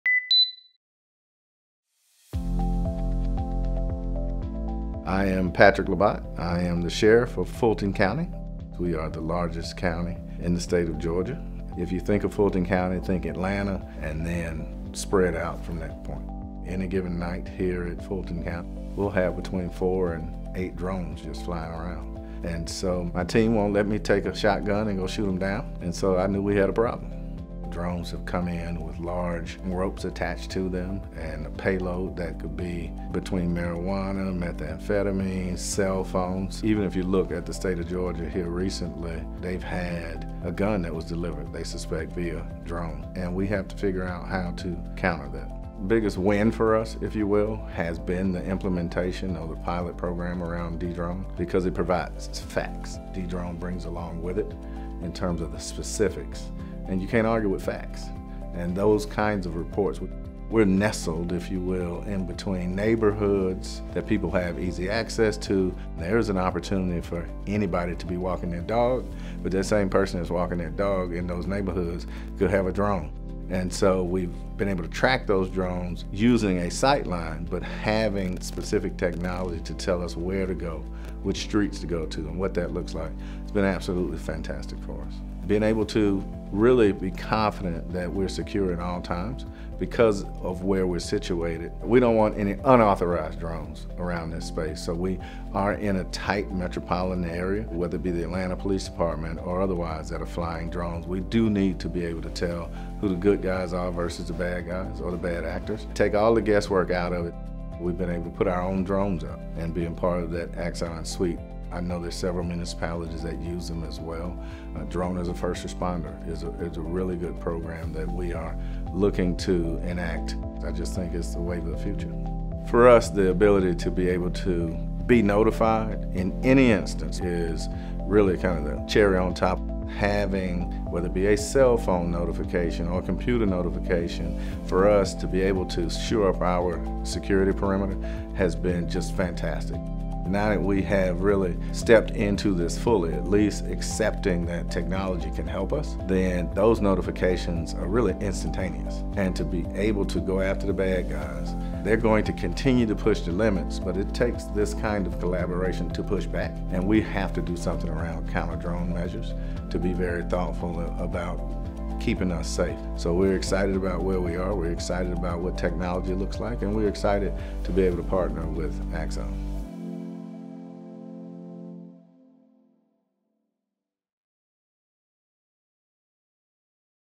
How Fulton County Jail is Combating Drone Threats with Dedrone by Axon
Discover how Fulton County Jail, Georgia's largest county detention facility, is proactively combating drone-delivered contraband using Dedrone's cutting-edge drone detection technology. Featuring Sheriff Patrick Labatt, this case study showcases how innovative solutions have enhanced safety, efficiency, and community protection, setting a new security benchmark for correctional facilities nationwide.